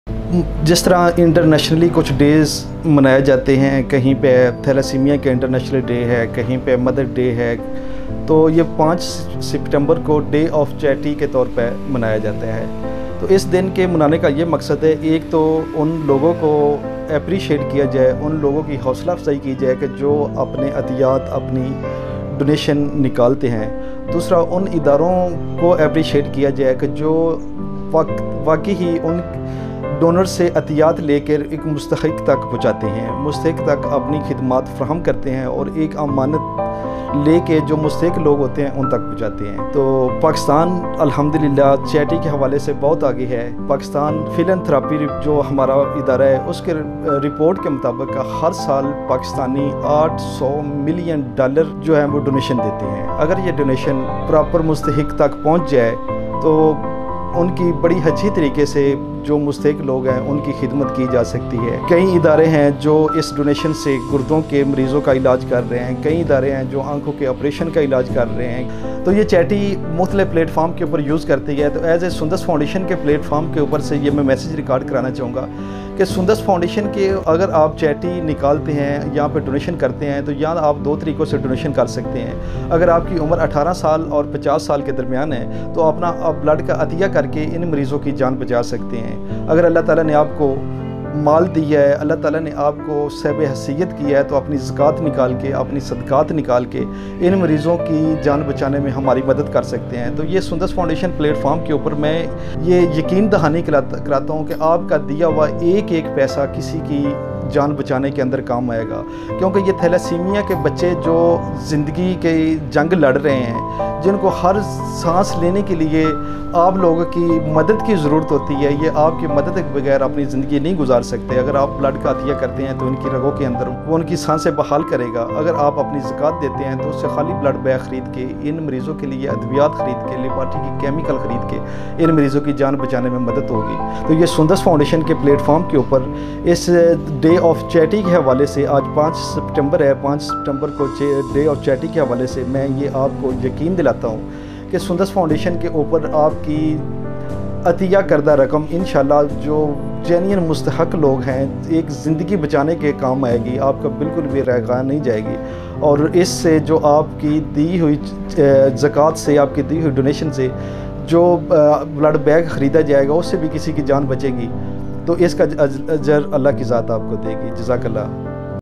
international day of charity | 5th September | Ghulam Nabi Ansari | Admin sundas foundation
5th September 🏵
International day of charity
The International Day of Charity was established with the objective of sensitizing and mobilizing people, NGOs, and stakeholders all around the world to to help others through volunteer and philanthropic activities.